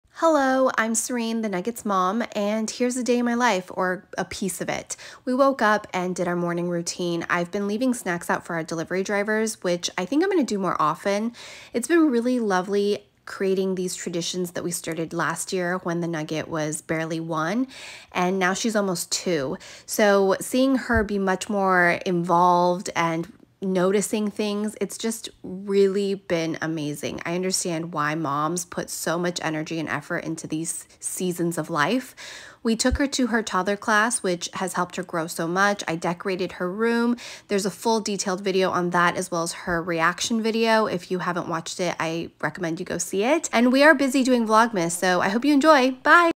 Day in My Life Vlog • Mom Life During The Holidays
Some mom moments and thoughts during the holidays with a toddler.